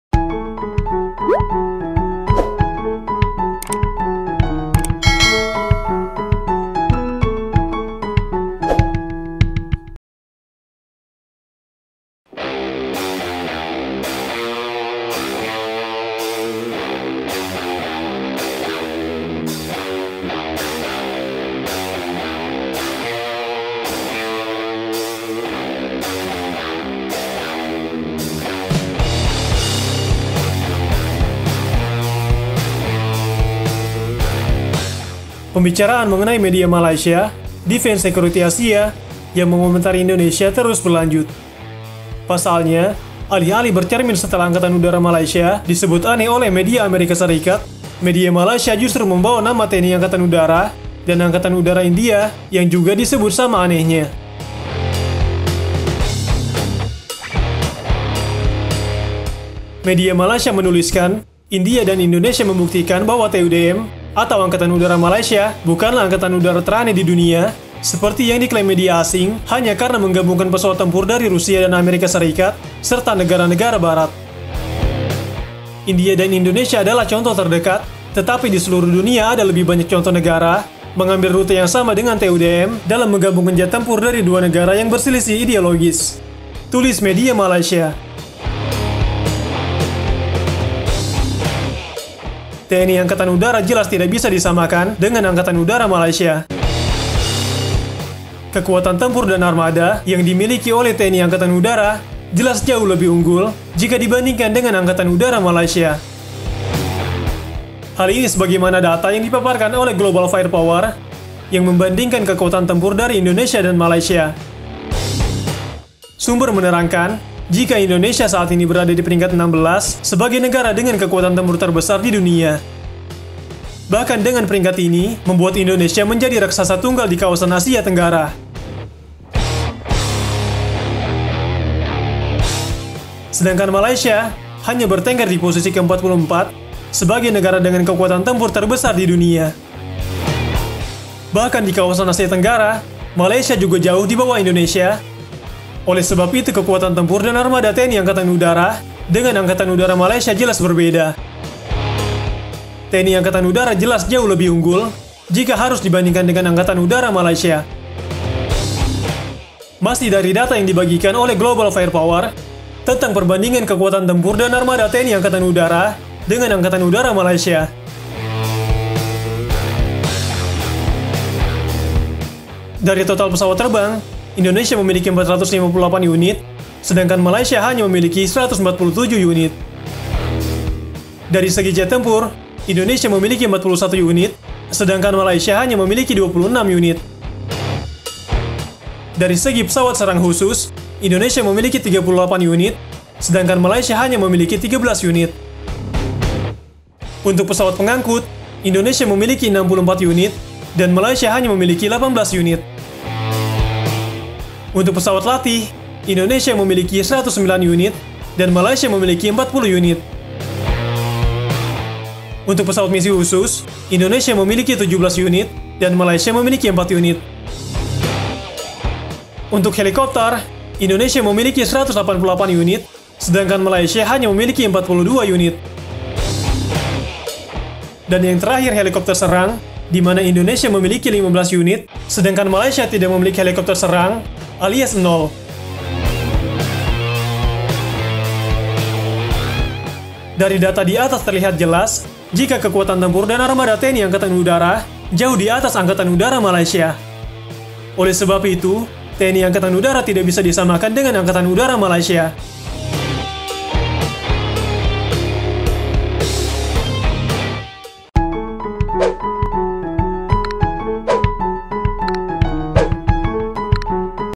MALAYSIA NEKAT SEBUT TNI ANGKATAN UDARA INDONESIA ANEH, LIHAT PERBANDINGAN KEKUATAN MILITER
Malaysia nekat sebut tni angkatan udara indonesia aneh, lihat perbandingan kekuatan militer alutsista udara kedua negara.

Pembicaran mengenai media Malaysia, Defence Security Asia yang mengomentari Indonesia, terus berlanjut.

Pasalnya, alih-alih bercermin setelah Angkatan Udara Malaysia disebut aneh oleh media AS, Forbes.

Media Malaysia justru membawa nama TNI AU dan Angkatan Udara India yang juga disebut sama anehnya.

"India dan Indonesia membuktikan bahwa TUDM (Angkatan Udara Malaysia) bukanlah angkatan udara teraneh di dunia seperti yang diklaim oleh penulis majalah Forbes, hanya karena menggabungkan pesawat tempur dari Rusia dan Amerika Serikat serta negara-negara Barat,” jelas Defence Security Asia.

“India dan Indonesia adalah contoh terdekat, tetapi di seluruh dunia ada lebih banyak contoh negara mengambil rute yang sama dengan TUDM dalam menggabungkan jet tempur dari dua yang berselisih ideologis," terangnya.

TNI AU jelas tak bisa disamakan dengan Angkatan Udara Malaysia. Kekuatan tempur dan armada yang dimiliki oleh TNI AU jelas jauh lebih unggul jika dibandingkan dengan Angkatan Udara Malaysia. Hal ini sebagaimana data yang dipaparkan oleh Global Fire Power yang membandingkan kekuatan tempur dari Indonesia dan Malaysia.

Sumber menerangkan jika Indonesia saat ini berada di peringkat 16 sebagai negara dengan kekuatan tempur terbesar di dunia. Bahkan dengan peringkat ini membuat Indonesia menjadi raksasa tunggal di kawasan Asia Tenggara. Sedangkan Malaysia hanya bertengger di posisi 44 sebagai negara dengan kekuatan tempur terbesar di dunia.

Bahkan di kawasan Asia Tenggara, Malaysia juga jauh di bawah Indonesia. Oleh sebab itu, kekuatan tempur dan armada TNI AU dengan Angkatan Udara Malaysia jelas berbeda. TNI AU jelas jauh lebih unggul jika harus dibandingkan dengan Angkatan Udara Malaysia.

Masih dari data yang dibagikan oleh Global Fire Power, untuk lengkapnya simak perbandingan kekuatan tempur dan armada TNI AU dengan Angkatan Udara Malaysia.

Perbandingan Alutsista Angkatan Udara Indonesia vs Malaysia

- Total pesawat terbang 458 vs 147
- Jet tempur 41 vs 26
- Pesawat serang khusus 38 vs 13
- Pesawat pengangkut 64 vs 18
- Pesawat latih 109 vs 40
- Pesawat misi khusus 17 vs 4
- Helikopter 188 vs 42
- Helikopter serang 15 vs 0

Dengan data di atas terlihat jelas jika kekuatan tempur dan armada TNI AU jauh di atas Angkatan Udara Malaysia. Oleh sebab itu, TNI AU tak bisa disamakan dengan Angkatan Udara Malaysia.